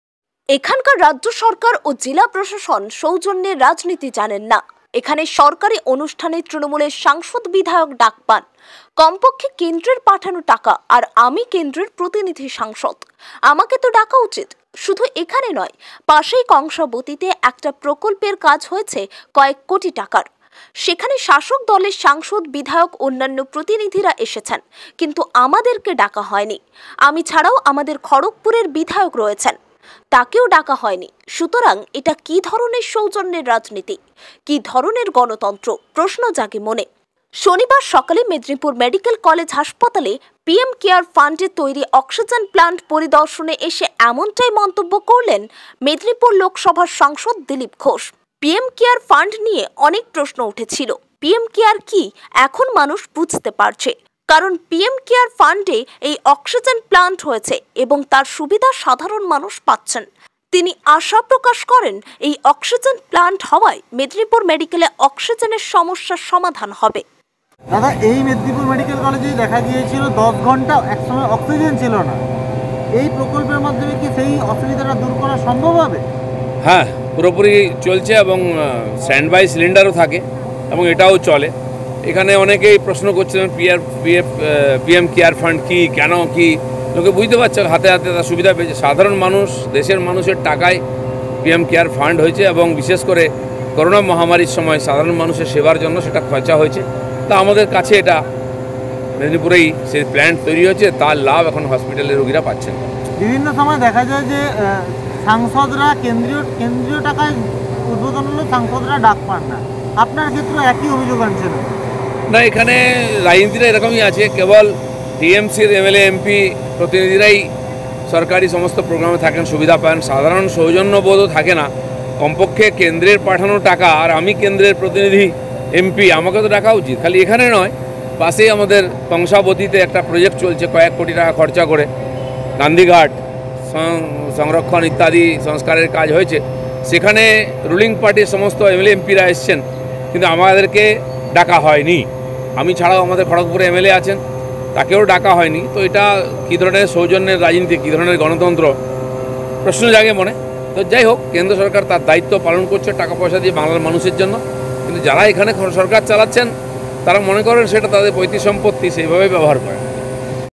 এখানকার রাজ্য সরকার ও জেলা প্রশাসন সৌজন্যের রাজনীতি জানেন না: দিলীপ ঘোষ |Oneindia Bengali
এখানকার রাজ্য সরকার ও জেলা প্রশাসন সৌজন্যের রাজনীতি জানেন না: দিলীপ ঘোষ